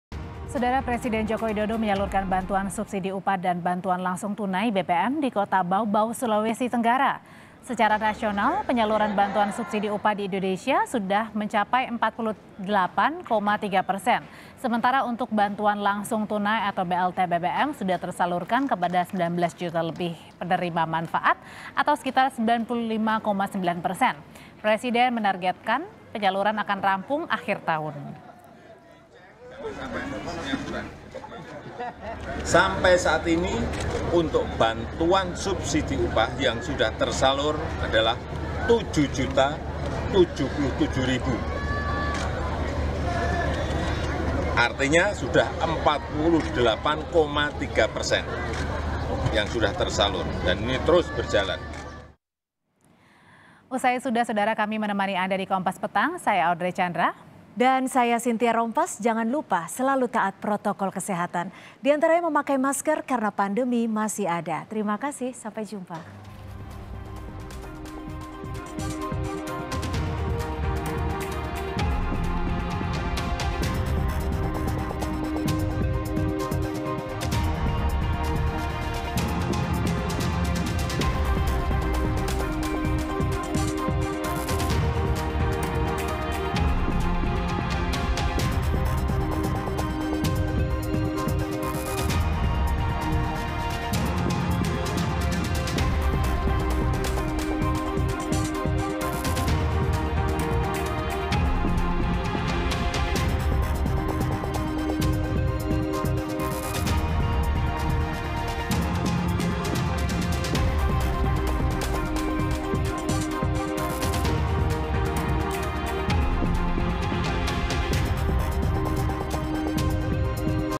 Jokowi Bagikan Secara Langsung BLT BBM & BSU di Baubau, Sulawesi Tenggara
BAUBAU, KOMPAS.TV - Presiden Joko Widodo menyalurkan Bantuan Subsidi Upah dan Bantuan Langsung Tunai BBM di Kota Baubau, Sulawesi Tenggara.

Secara nasional penyaluran Bantuan Subsidi Upah di indonesia sudah mencapai 48,3%.

Baca Juga Lakukan Ini bila Ingin Cek Nomor BPJS Ketenagakerjaan tapi Kartunya Hilang atau Rusak di

Sementara untuk bantuan langsung tunai atau BLT BBM, sudah tersalurkan kepada 19 juta lebih orang penerima manfaat atau 95,9%.

Presiden menargetkan penyaluran akan rampung akhir tahun ini.